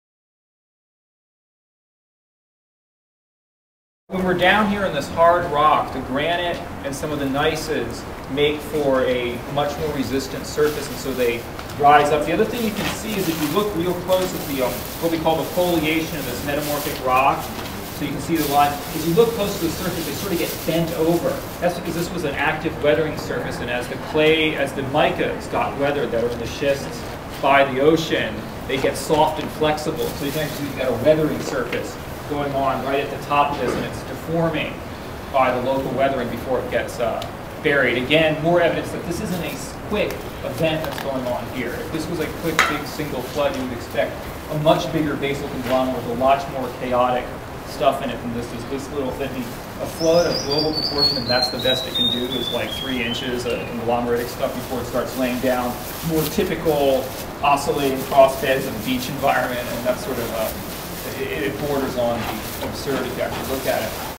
An unconformity in the Grand Canyon--Loud version
NCSE's Dr. Alan Gishlick explains an unconformity in the Grand Canyon